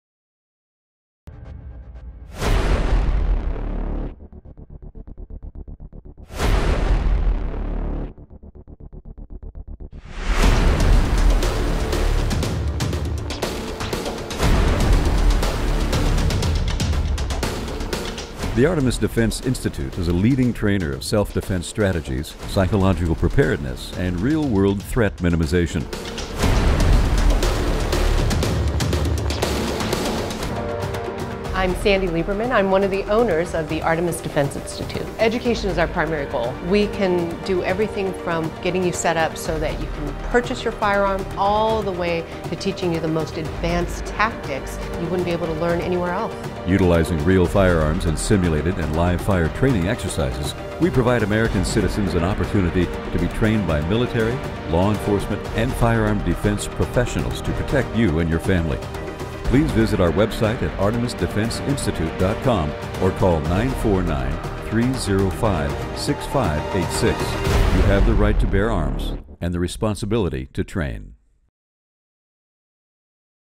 Artemis Defense Institute's Overview
Here's an overview of what we do.  We welcome all levels of abilities... and trust that you will walk away learning something new about yourself!